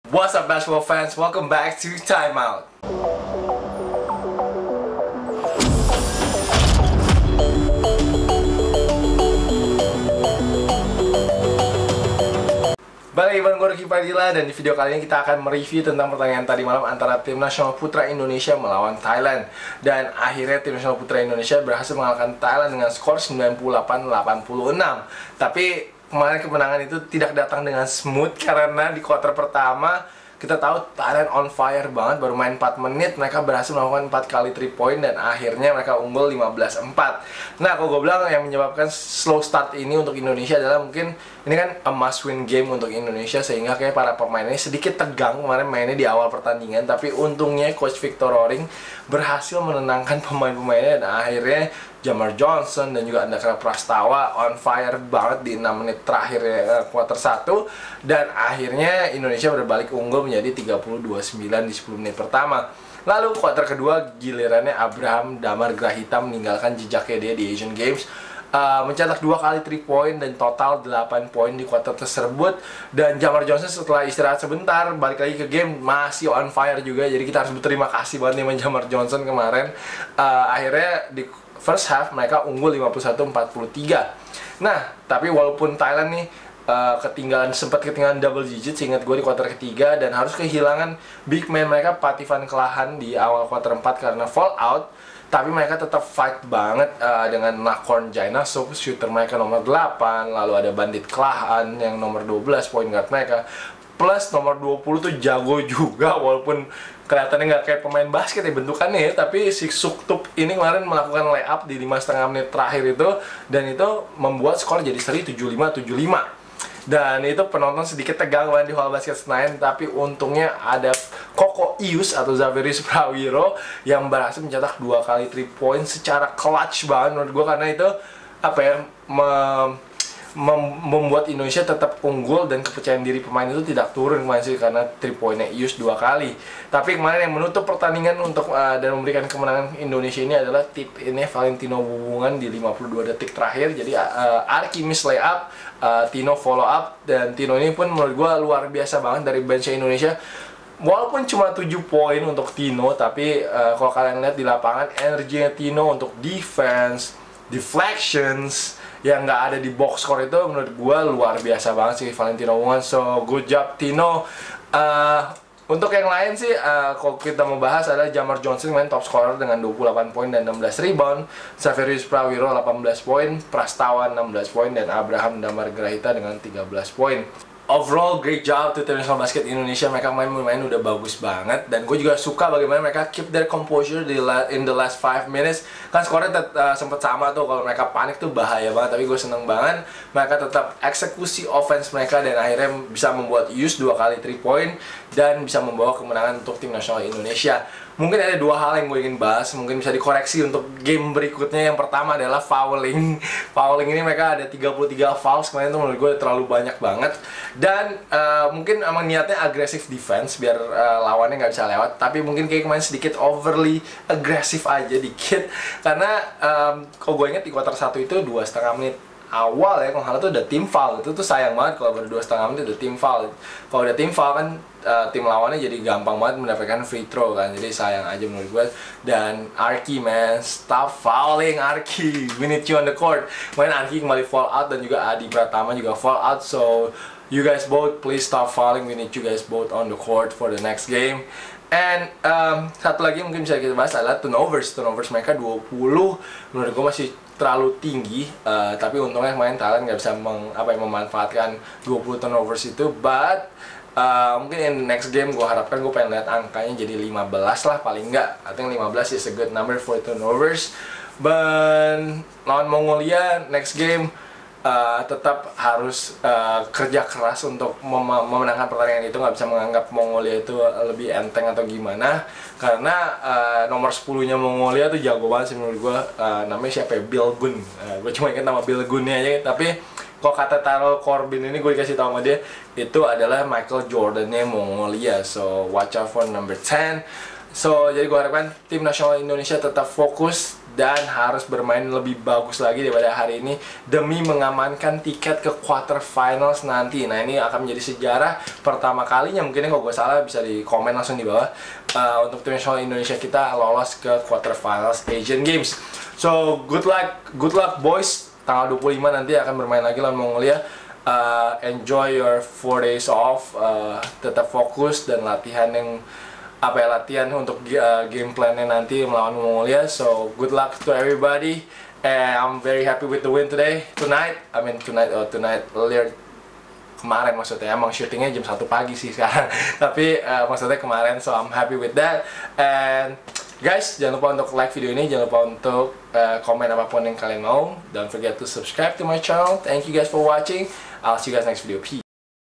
Time Out #162: Tim Nasional Putra Asian Games Kalahkan Thailand!
Short review dari pertandingan Asian Games Indonesia lawan Thailand.